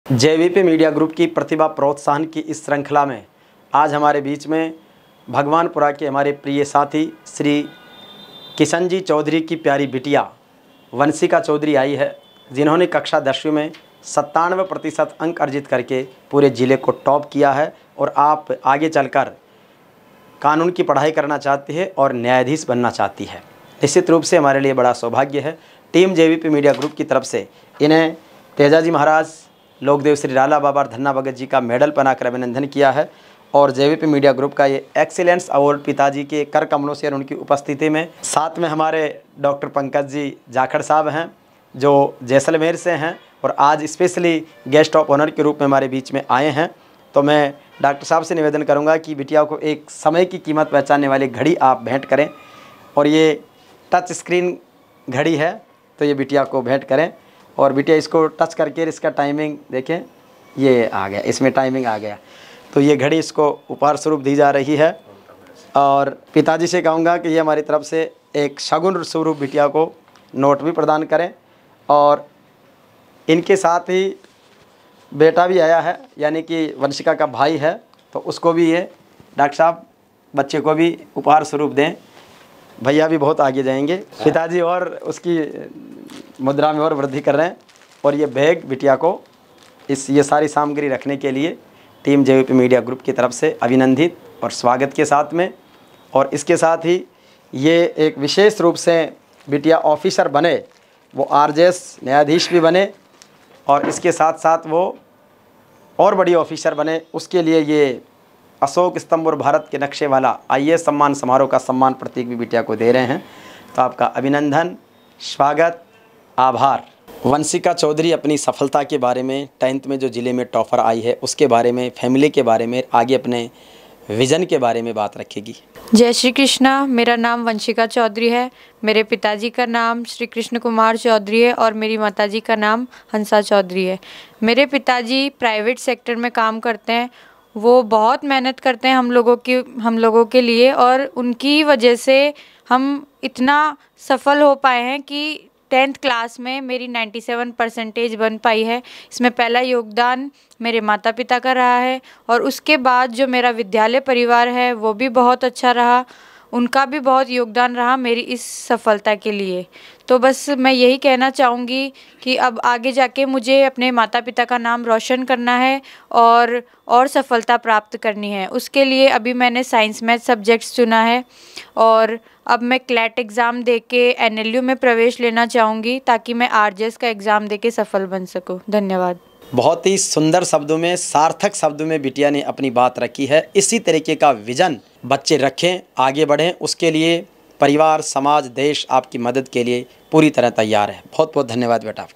टोंक जिला टॉपर वंशिका चौधरी के ऑफिसर बनने का संकल्प और जेवीपी मीडिया ग्रुप का एक्सीलेंस अवॉर्ड
Tonk district topper Vanshika Chaudhary's determination to become an officer and JVP Media Group's Excellence Award.
कक्षा 10 में टोंक जिला टॉपर वंशिका चौधरी को जेवीपी मीडिया ग्रुप का एक्सीलेंस अवार्ड भेंट करते हुए हर्ष और गौरव की अनुभूति है। वंशिका चौधरी कानून की पढ़ाई कर न्यायाधीश बनाना चाहती है। जेवीपी मीडिया समूह वंशिका चौधरी के लक्ष्य प्राप्ति में यथोचित सहयोग और सम्मान के लिए प्रतिबद्ध है। वंशिका के सम्मान समारोह में उनके पिताजी किशन कुमार चौधरी, भाई अभिराज और हमारे विशेष मेहमान डॉ. पंकज जाखड़ मौजूद रहे।  जय हो तेजल तपधारी जय श्री रालाबाबा धाम किंवाड़ा ।
Congratulations To Vanshika Choudhary for Excellence Performance in class 10th.
With regards Hariram kiwada Chairman JVP Media Group